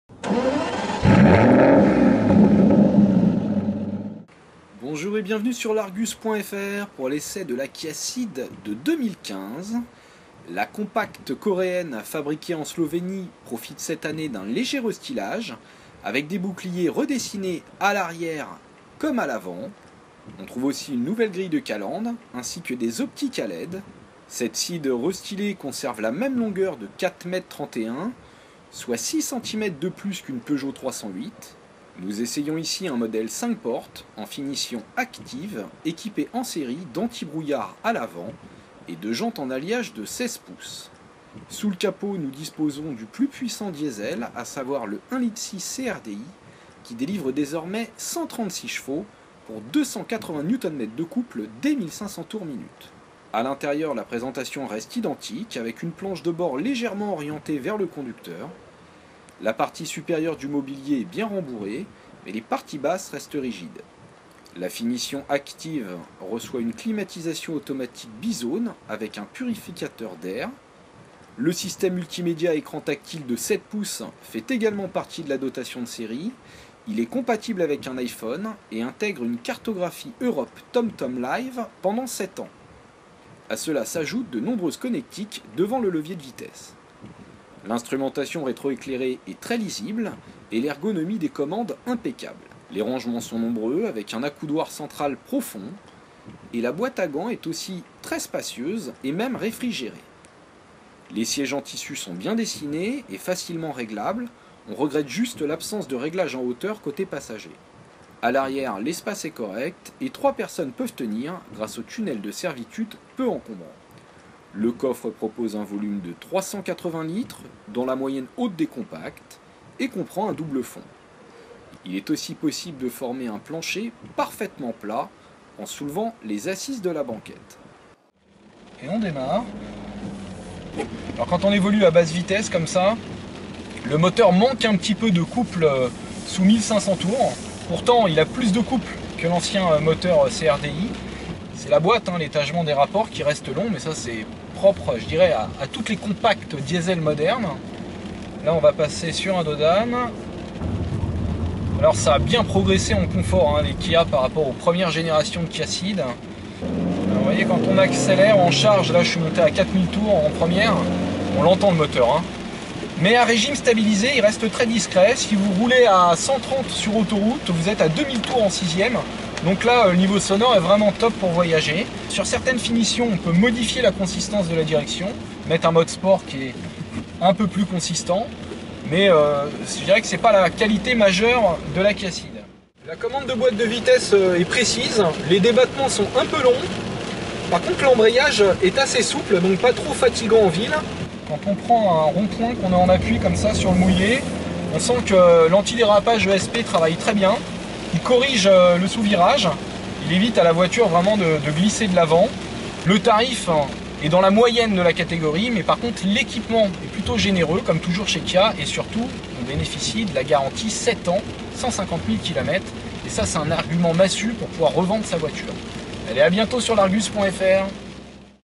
Essai Kia cee'd 1.6 CRDi ACTIVE 2015 - L'argus fr
Vidéo au volant de la nouvelle Kia cee'd animée par le moteur diesel 1.6 CDRi de 136 ch.